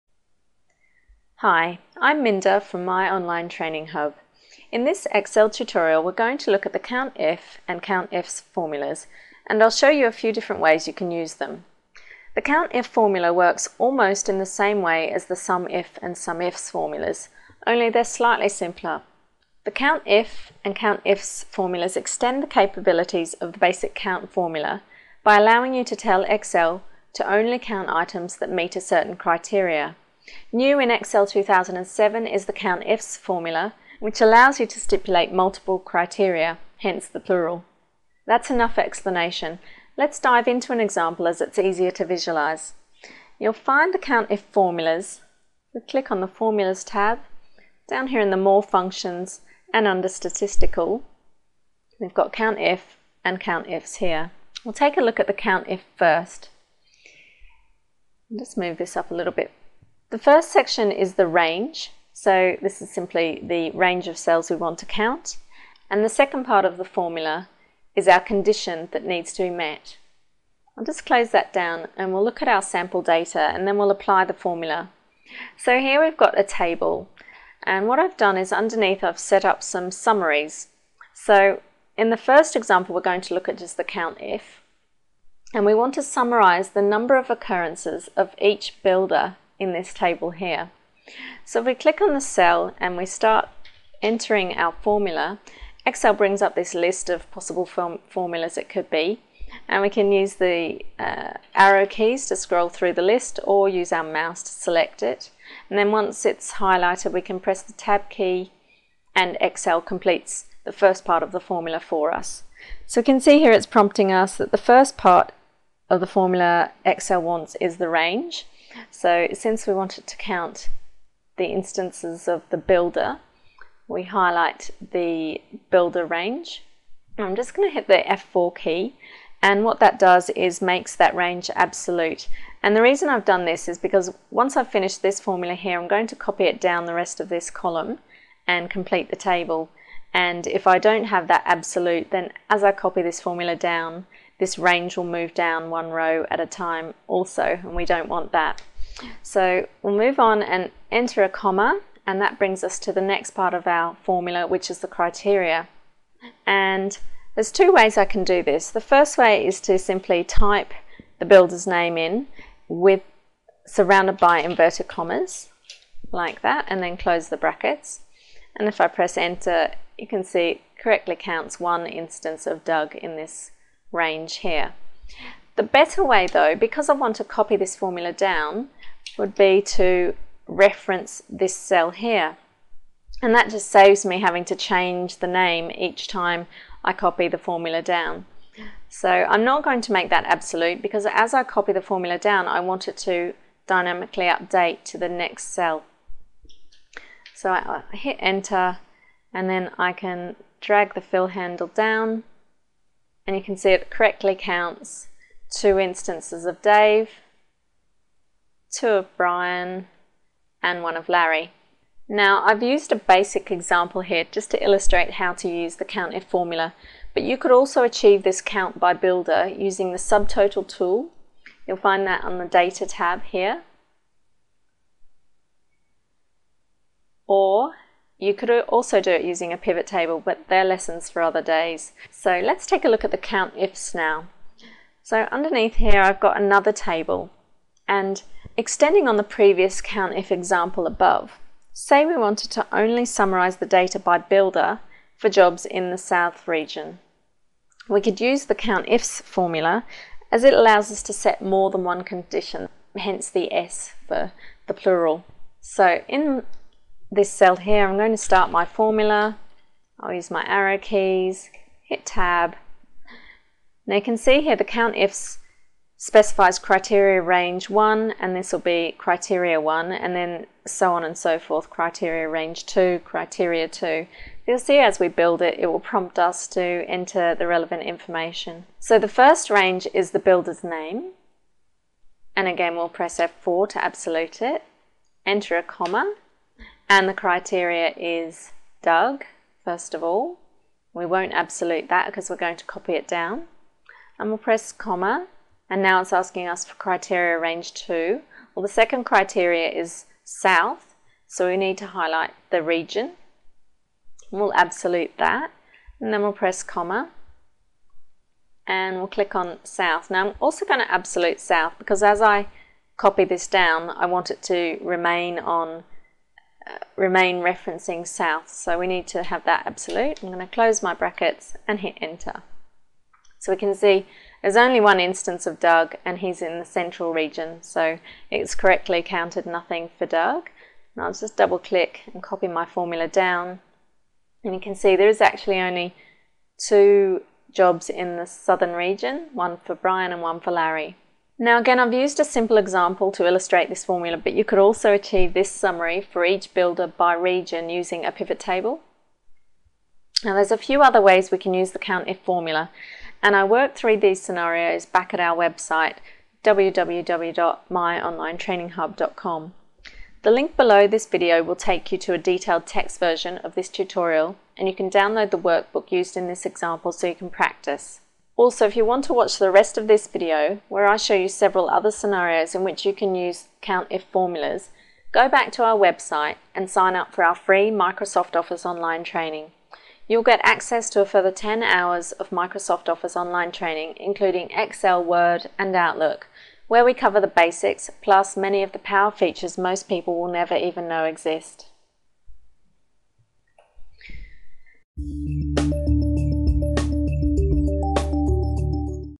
Excel COUNTIF & COUNTIFS Video Tutorial
Learn how to use COUNTIF and COUNTIFS formulas in Excel. Go here for step by step written instructions